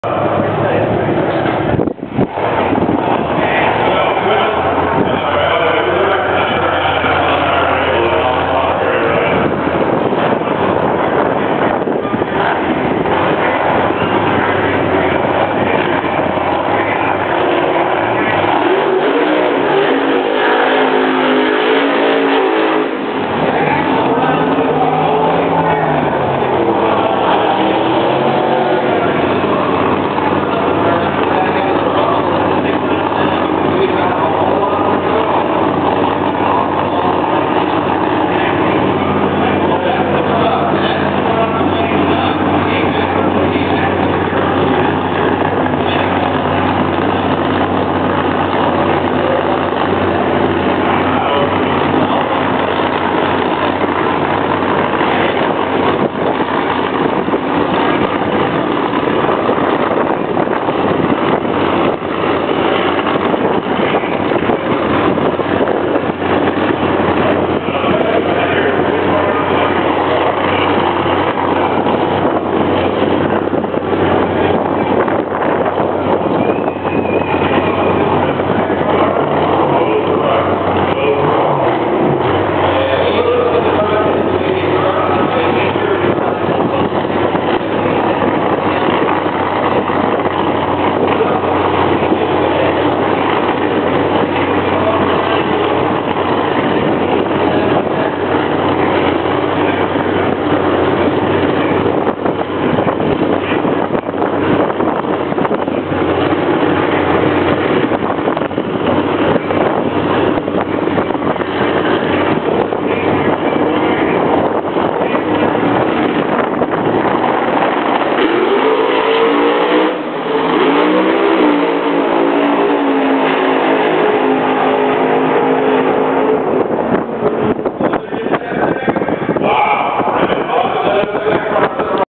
2006 PSCA Mike Moran vs. Garrett Richards 1st rd. elim.
Easy win for Mike Moran as he advances to 2nd round of eliminations to compete against L. Smith.

W- Mike Moran 7.350@200.41
L- Garrett Richards 7.687@184.37